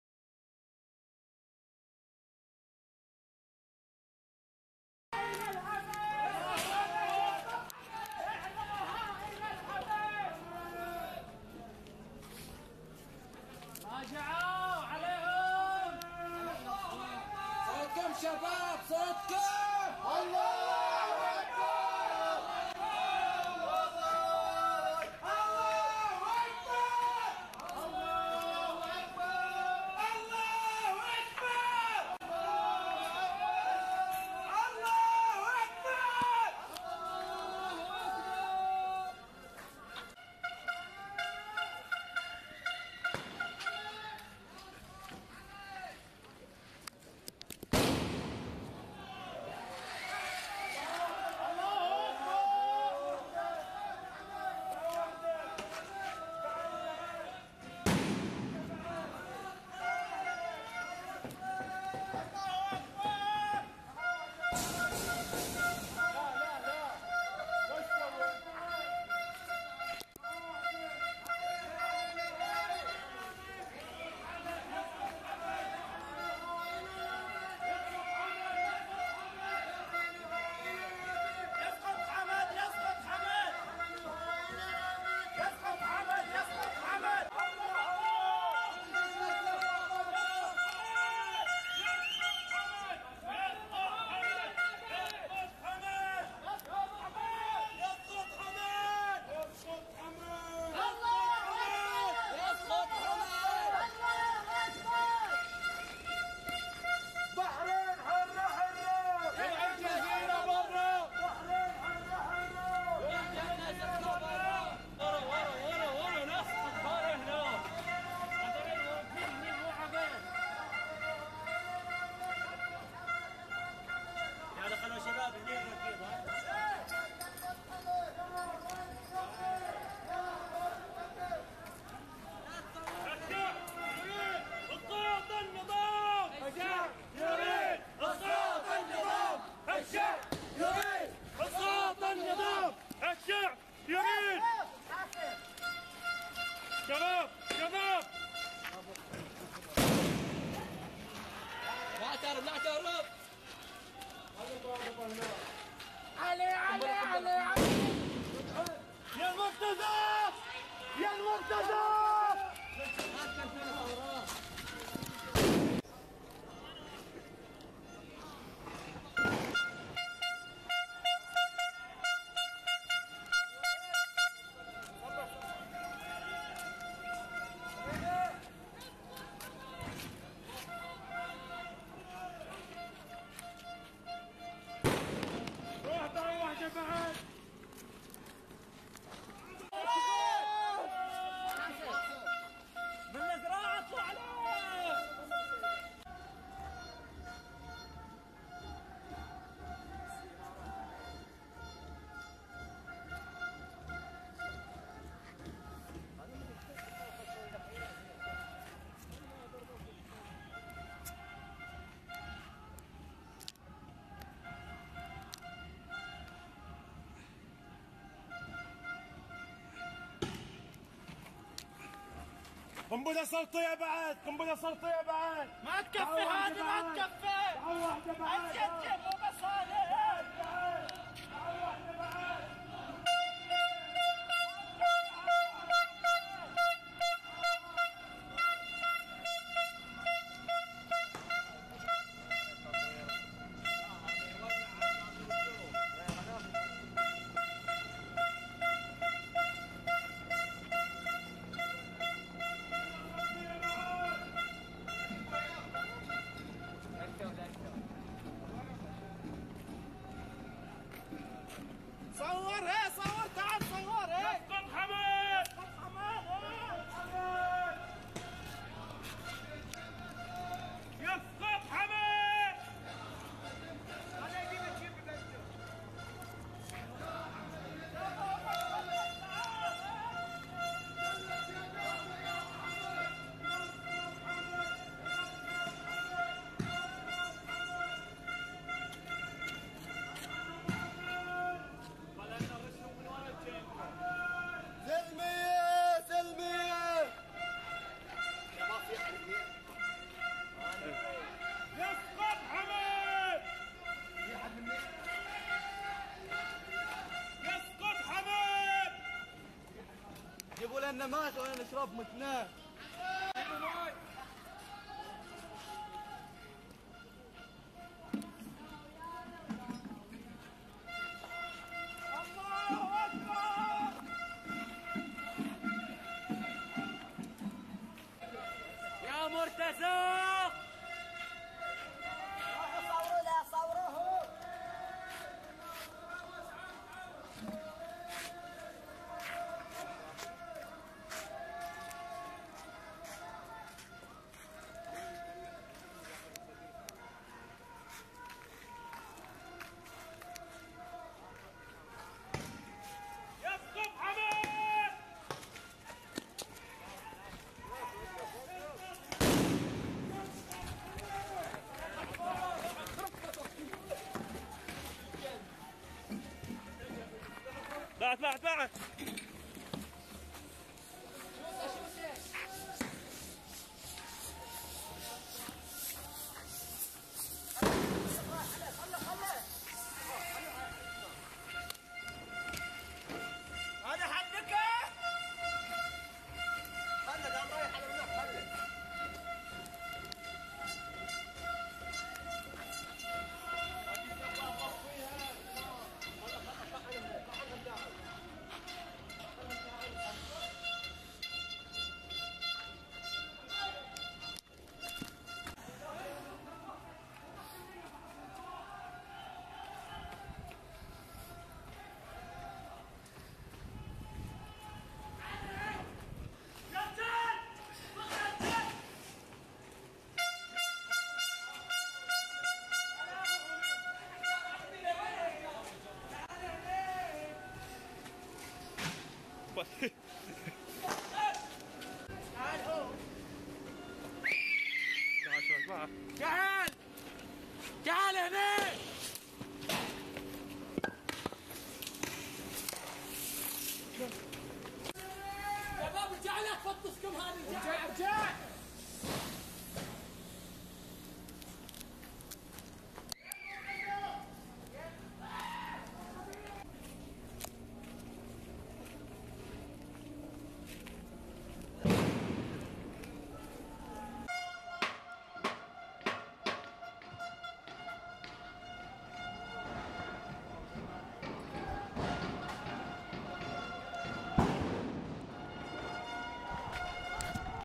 أحداث قرية أبوصيبع ذكرى الأستقلال 4/2
18/8/2011 أحداث قرية أبوصيبع في الذكرى السنوية للأستقلال الجزء الثاني